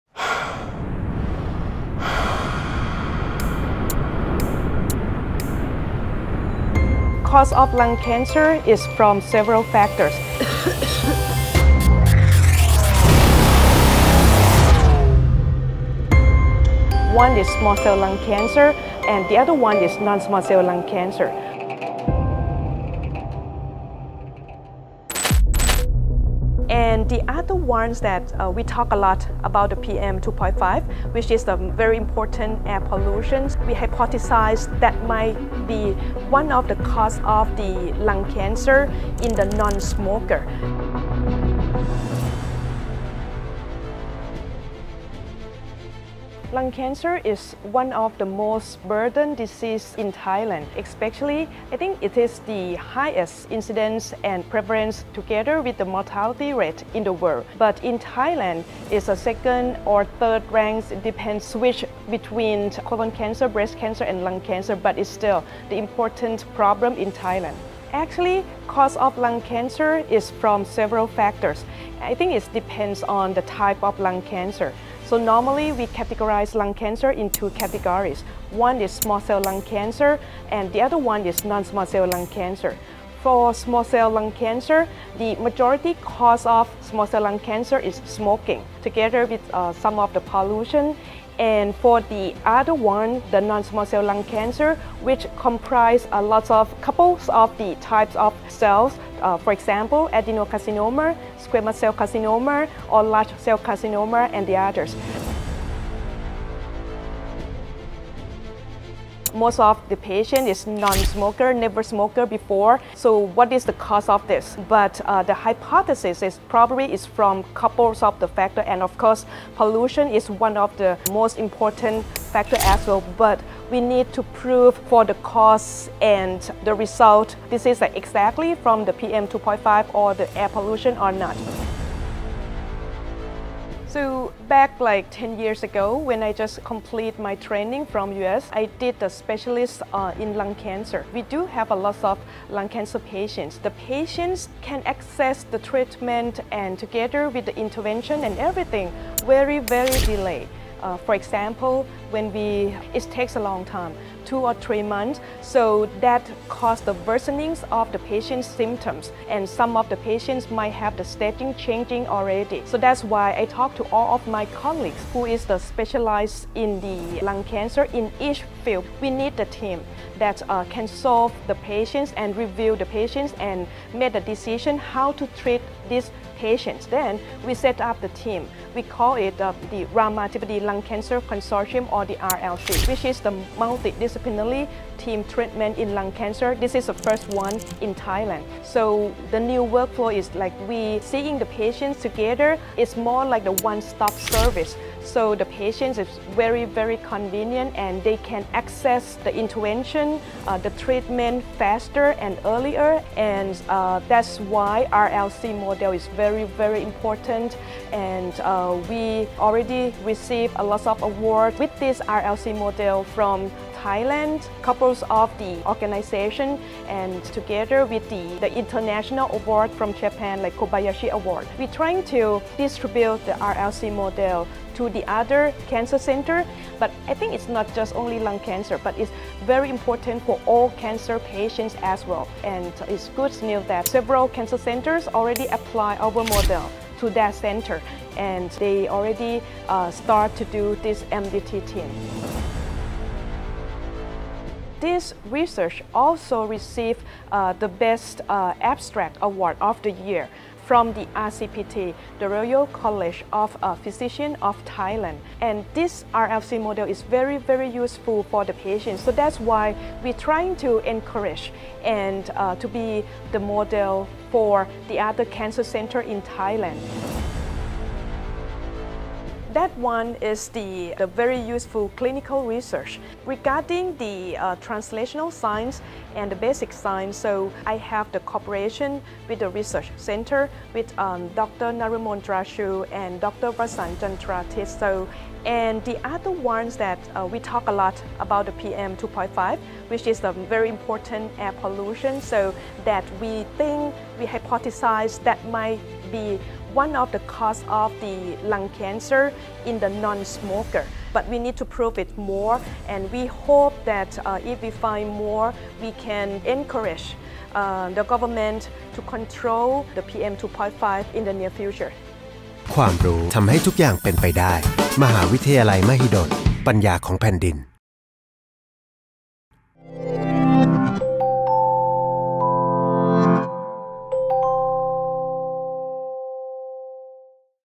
RLC Model: A Game Changer in Lung Cancer Treatment | Research Impact
Lung cancer is one of the most burdened diseases in Thailand. The cause of lung cancer comes from several factors. Most of the patients is non-smoker, never smokers before. And, of course, pollution is one of the most important factors as well. But we need to prove the cause and the result, which is exactly from the PM2.5 or the air pollution or not.

มะเร็งปอดเป็นหนึ่งในโรคที่สร้างภาระทางสุขภาพมากที่สุดในประเทศไทย สาเหตุของมะเร็งปอดเกิดขึ้นได้จากหลายปัจจัย ที่น่าสนใจคือ ผู้ป่วยส่วนใหญ่ที่เป็นมะเร็งปอดนั้น ไม่เคยสูบบุหรี่มาก่อน แน่นอนว่ามลพิษทางอากาศก็เป็นหนึ่งในปัจจัยสำคัญ อย่างไรก็ตาม เรายังจำเป็นต้องมีหลักฐานที่ชัดเจนเพื่อพิสูจน์ถึงความสัมพันธ์ระหว่างสาเหตุและผลลัพธ์ ว่ามะเร็งปอดเกิดจาก PM2.5 หรือมลพิษทางอากาศโดยตรงหรือไม่?